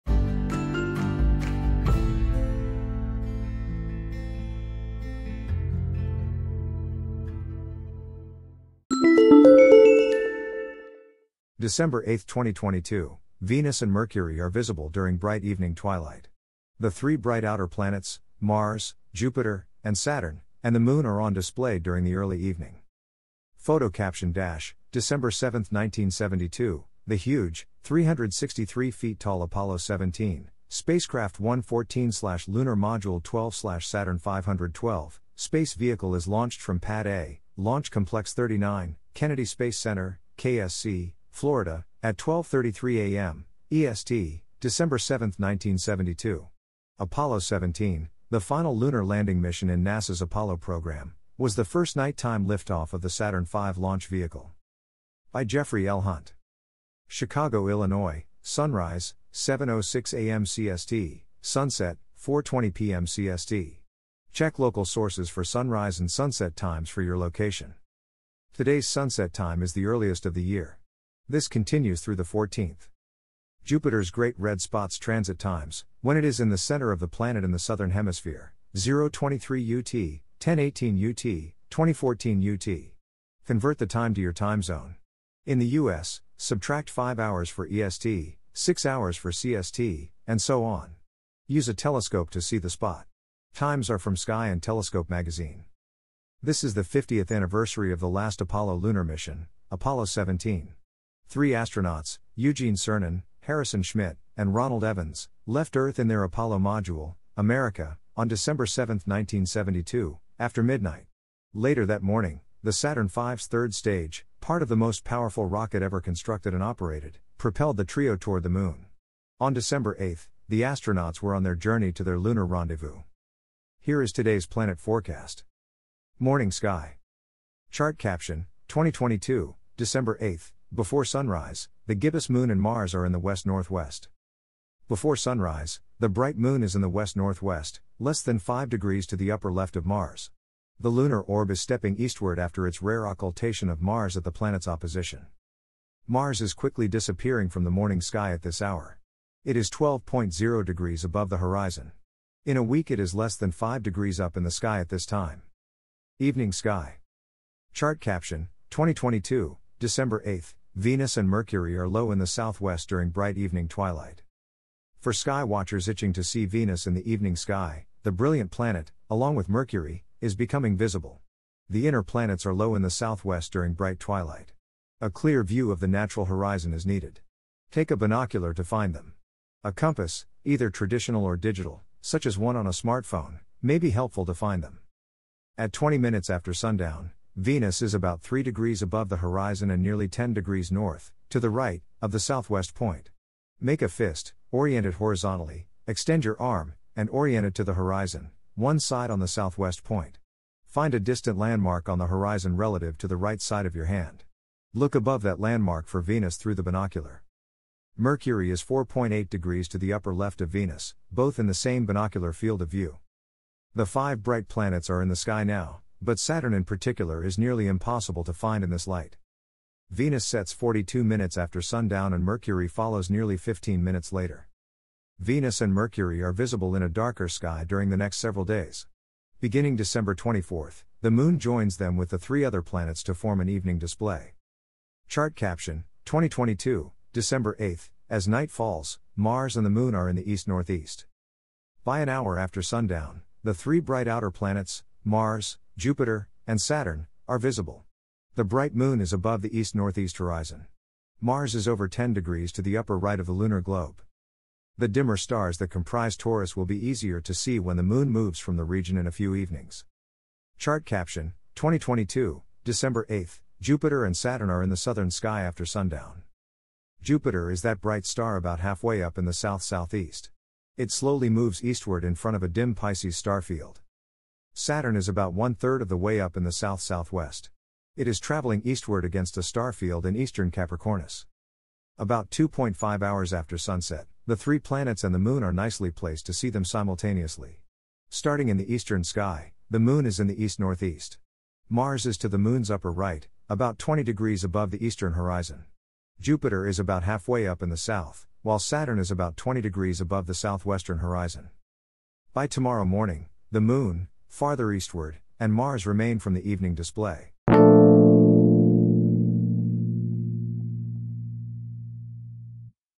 2022, December 8: Early Views, Venus, Mercury, Bright Evening Planets
December 8, 2022: Venus and Mercury are visible during bright evening twilight.  The three bright outer planets – Mars, Jupiter, and Saturn – and the moon are on display during the early evening.

This episode is also available as a blog post: 2022, December 8: Early Views, Venus, Mercury, Bright Evening Planets